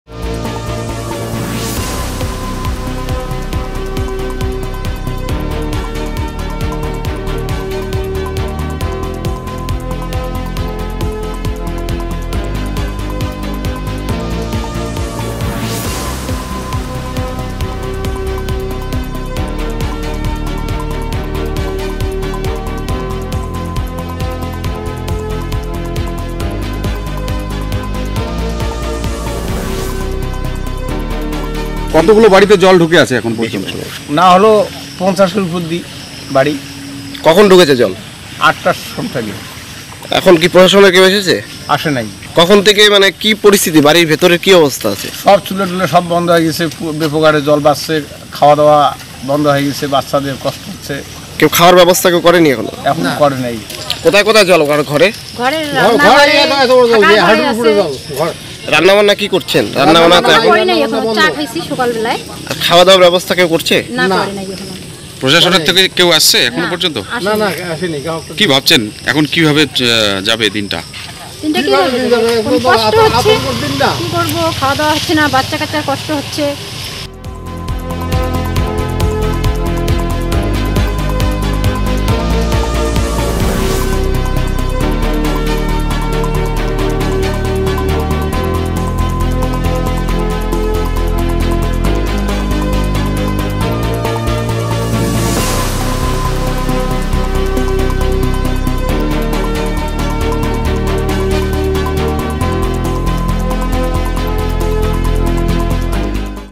Alipurduar Flood: Army Deployed to Rescue Trapped People in Alipurduar During Flood
Alipurduar Flood: Army deployed to rescue trapped people in Alipurduar during the flood.

Non-stop heavy rainfall devastates the situation of Alipurduar. The army has started rescue work in water-logged Dooars. The locals suffer from food shortages & stuck inside the house due to the water being clogged.